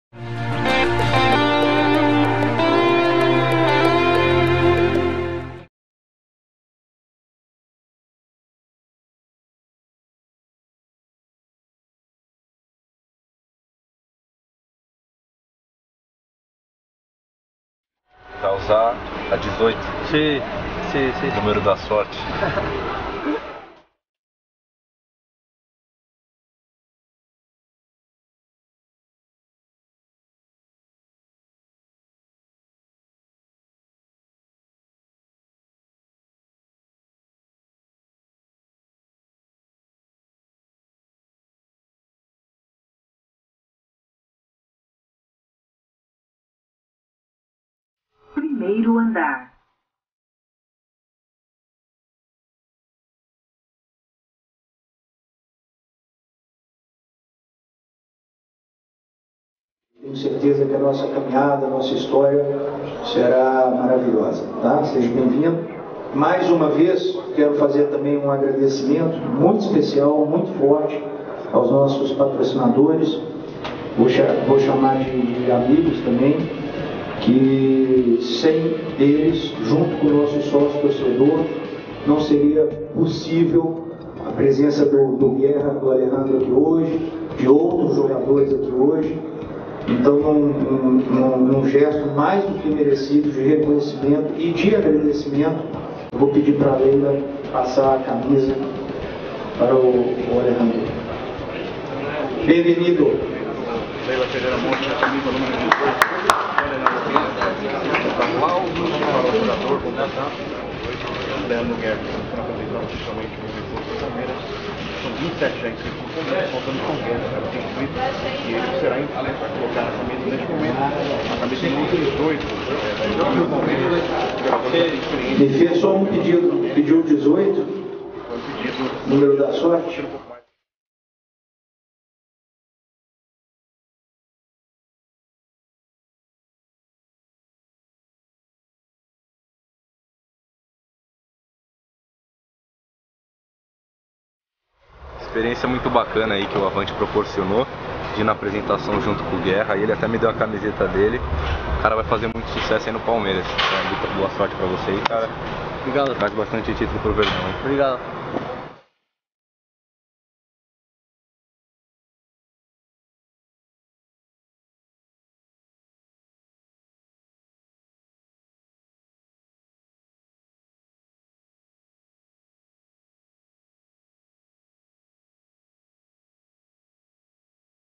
Sócio Avanti participa da apresentação do Guerra do palmeiras - 02/02/2017 - HD
Sócio Avanti Kaike Duarte participa da apresentação do Guerra do palmeiras - 02/02/2017 - HD

Video da TV PALMEIRAS/FAM